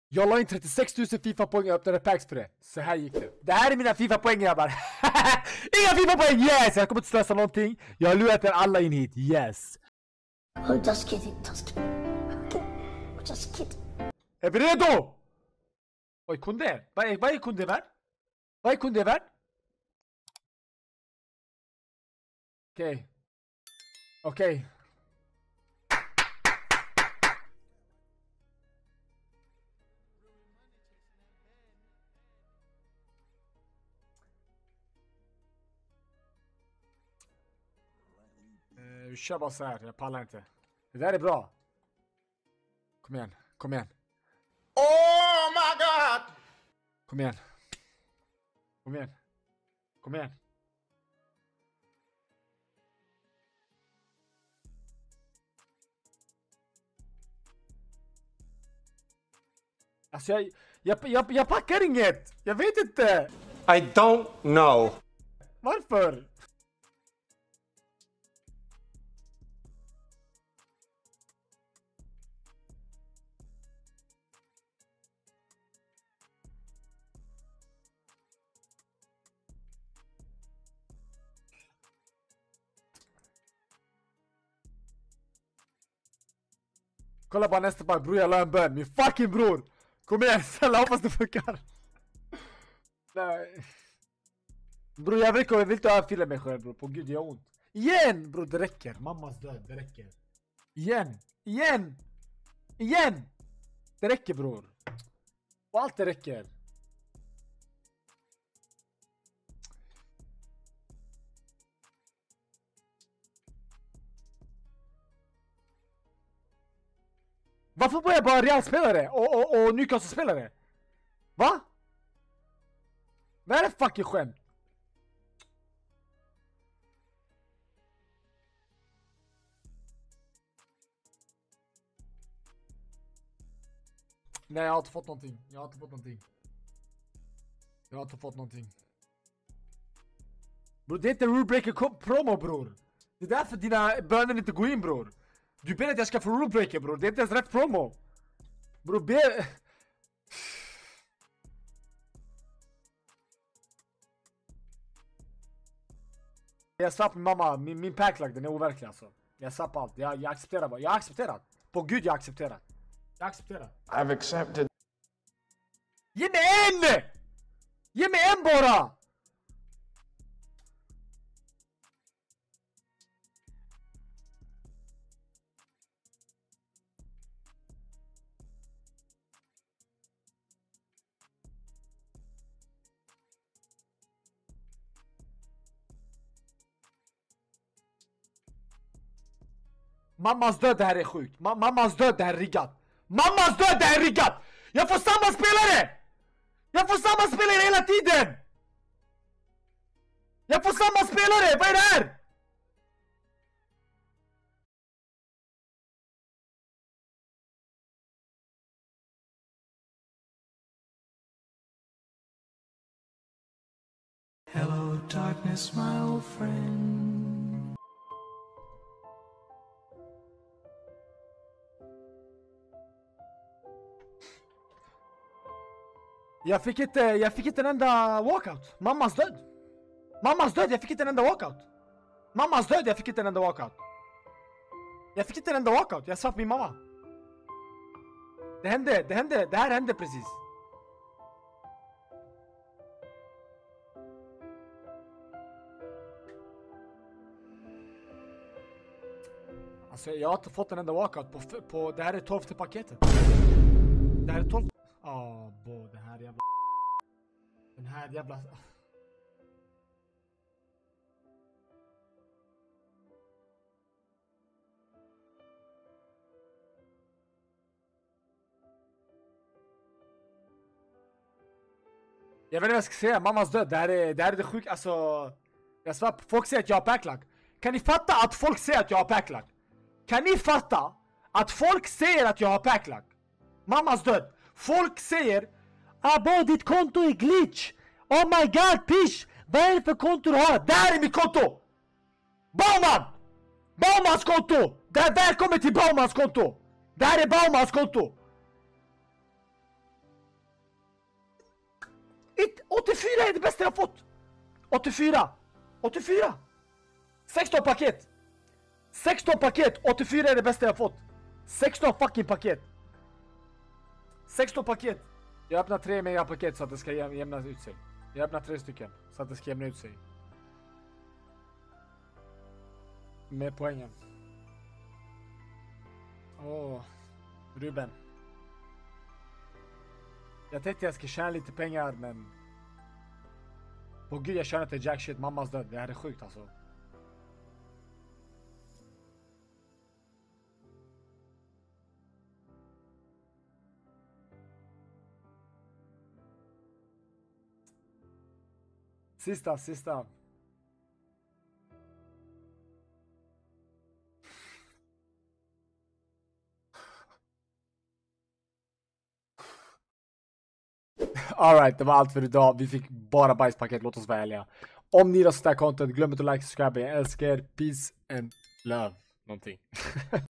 JAG LA IN 36 K FP FÖR ATT PACKA EN OUT OF POSITION SPELARE!
Vi öppnar Paket för massa massa FP! Hur många Rulebreakers kan man packa??